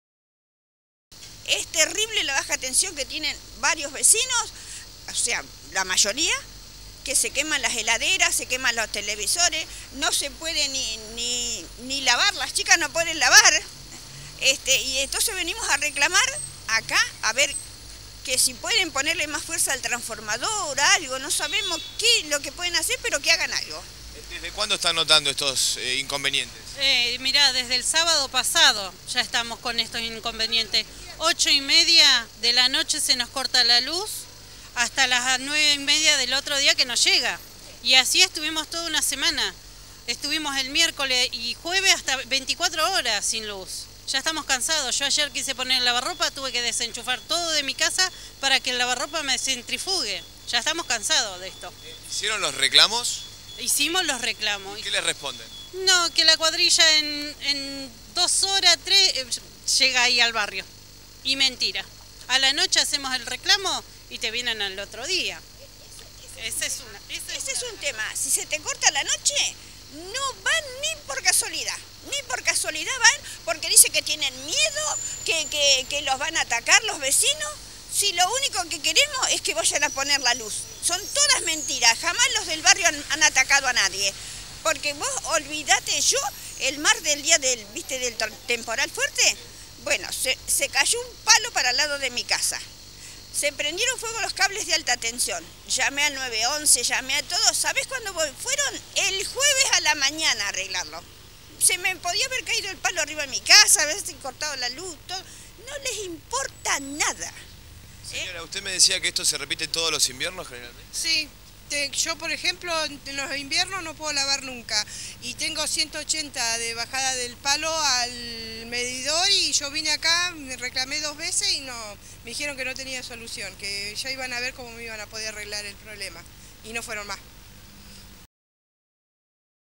Protesta contra EDES
Por cortes y baja tensión en el barrio 9 de Noviembre.